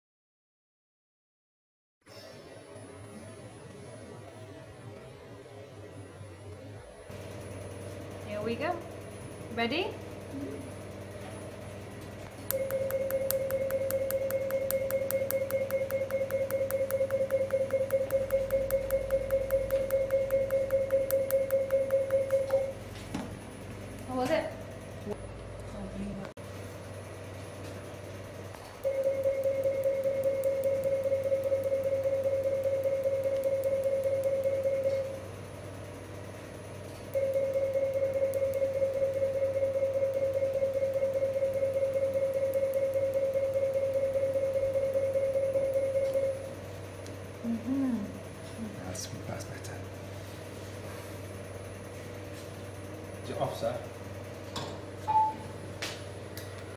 Laser Tattoo Removal Dubai By Our Specialist Doctor @ Euromed Clinic Dubai
Do you want to get rid of ugly tattoo? Then you have to consult with us and we'll remove the ink with latest technologies with no side effects.